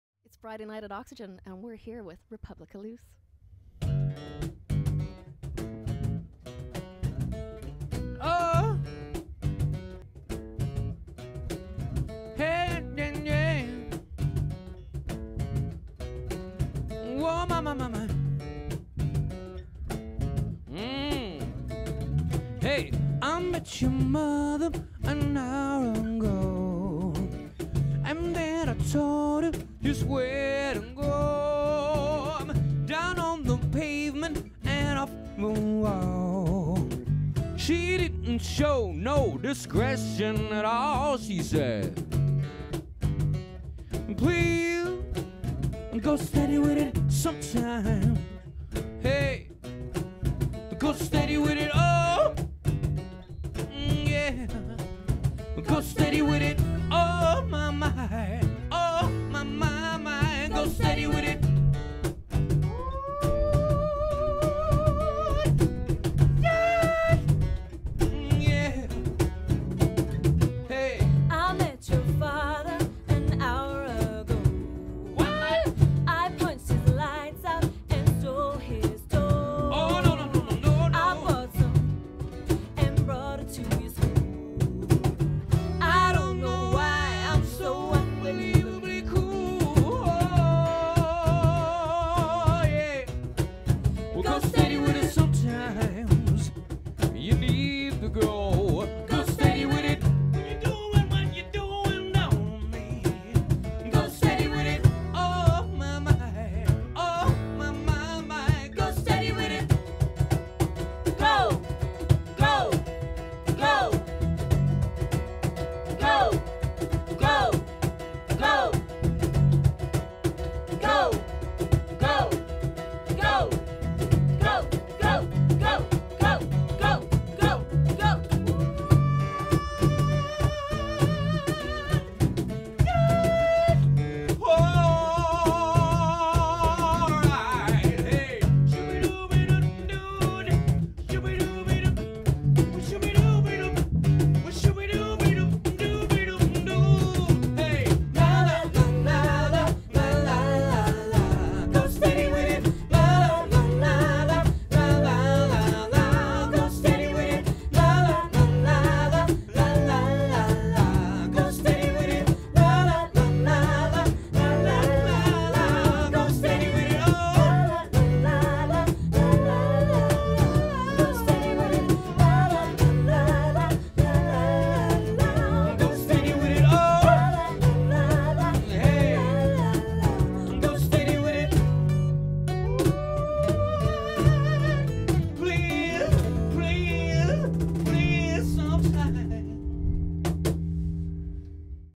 Oxegen 09 Republic of Loose
Republic of Loose in an exclusive 2fm Green Room Session